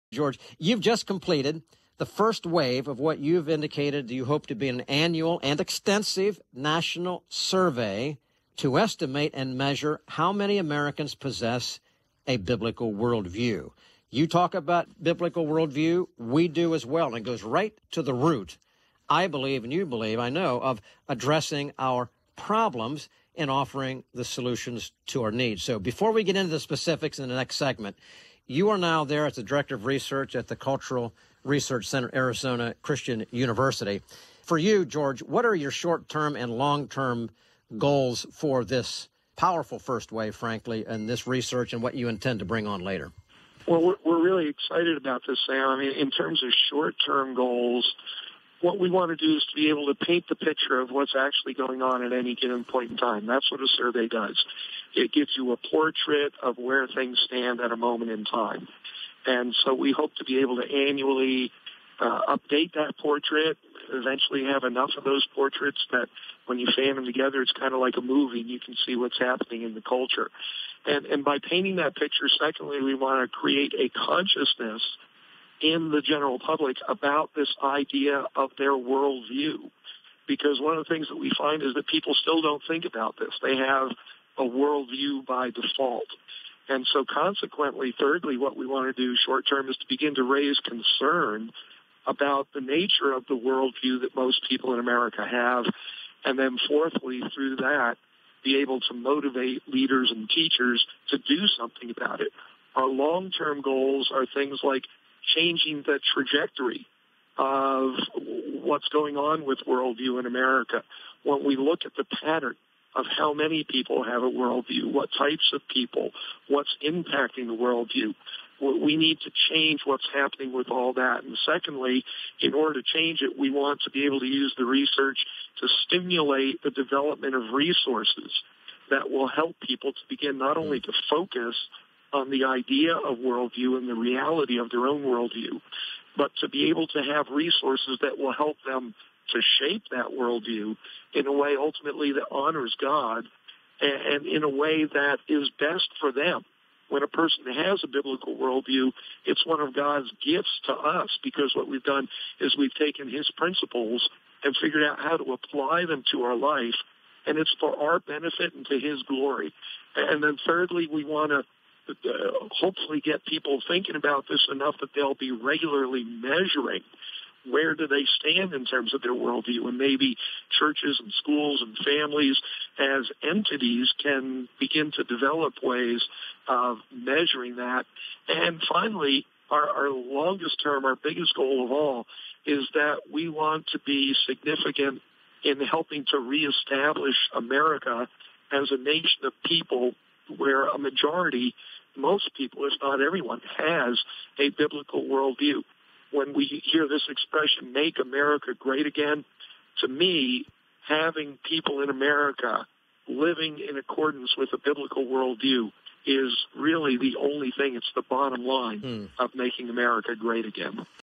The Latest from George Barna
George Barna (Author, Speaker, Founder - The Barna Group) has released new research titled, “Dangerously Few Americans Possess a Biblical Worldview”. George hopes this study will be an annual (and extensive) survey to estimate and measure how many Americans possess a Biblical Worldview. In this clip George shares the short and long term intent for this research.

Speakers: Sam Rohrer, George Barna